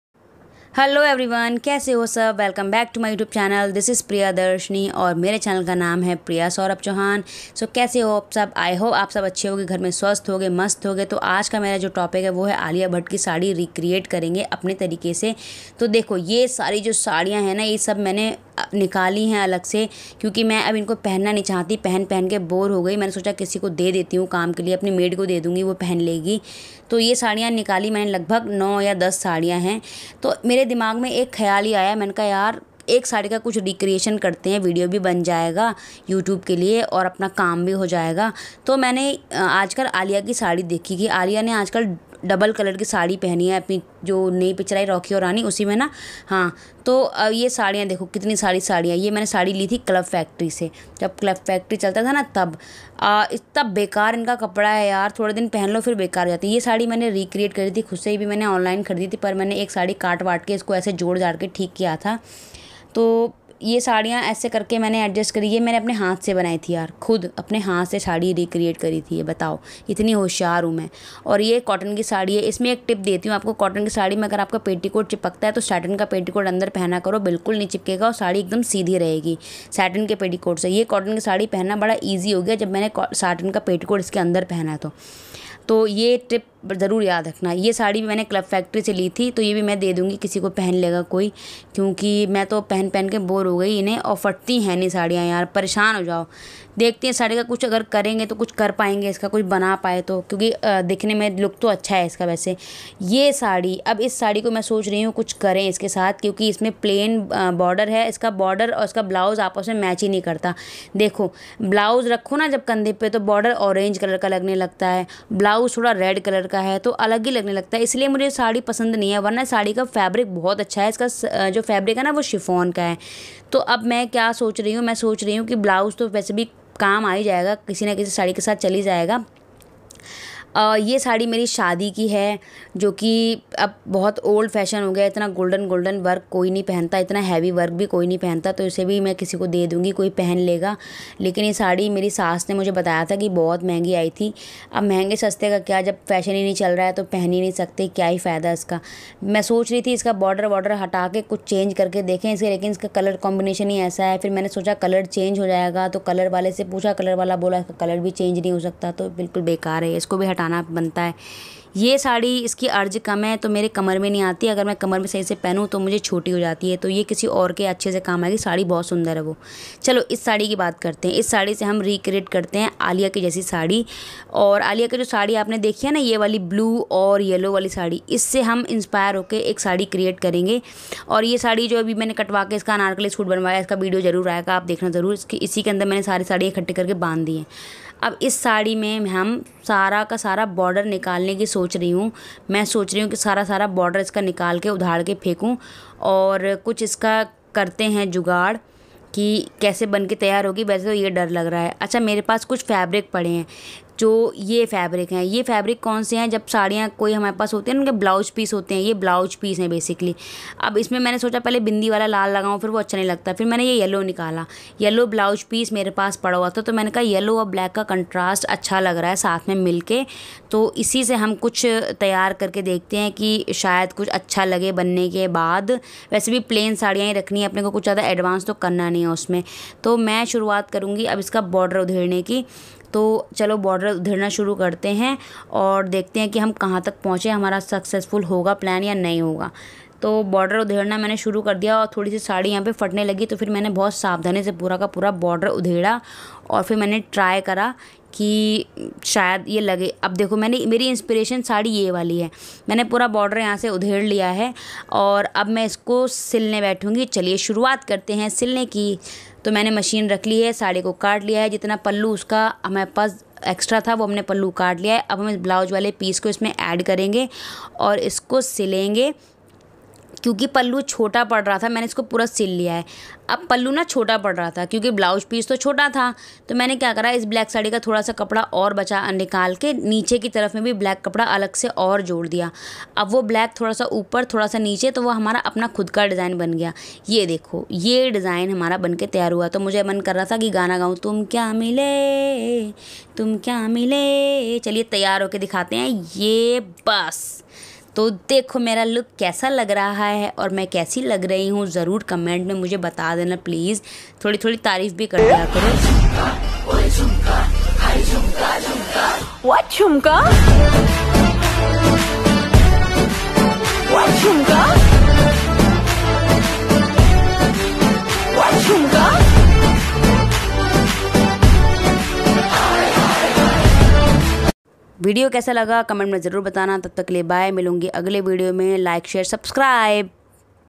saree reuse idea / recreate alia bhatt design saree with old saree / पुरानी साड़ी से बनाएं नई साड़ी
saree from scratch / let's recreate alia's double color plain saree / पुरानी साड़ी से बनाएं नई साड़ी